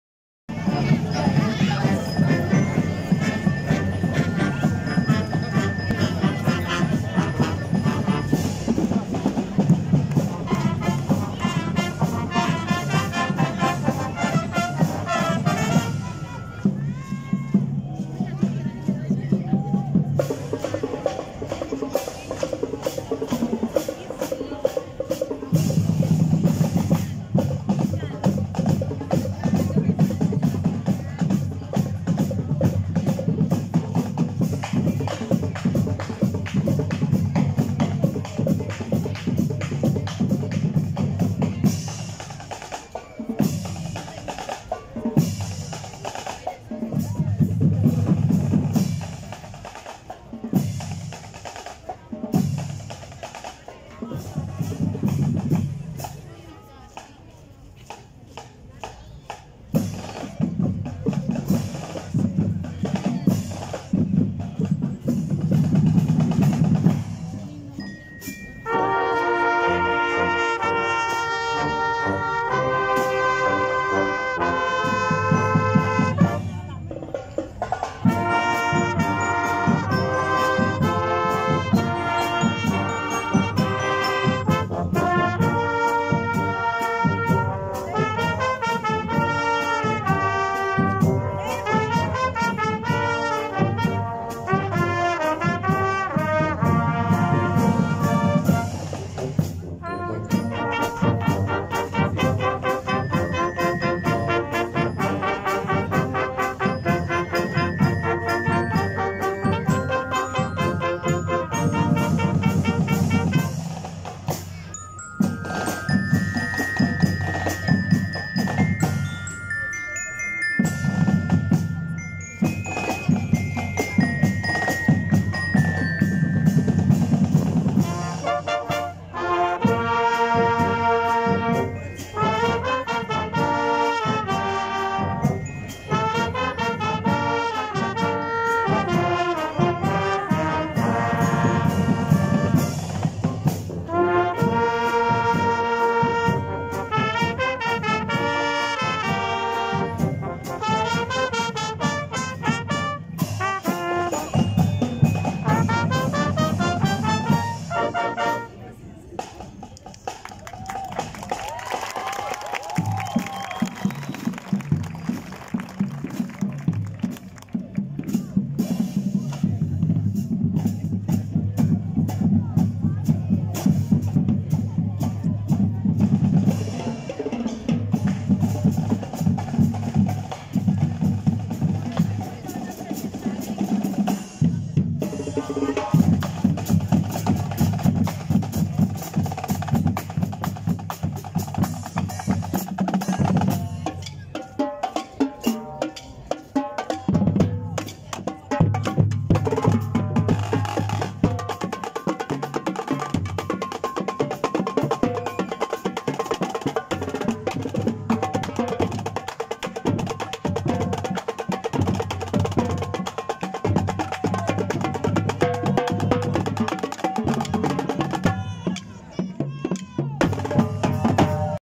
Banda Instituto Técnico Industrial de Tocancipá | Concurso Facatativá 2022
Categoria Open, Concurso Wambs Facatativá 2022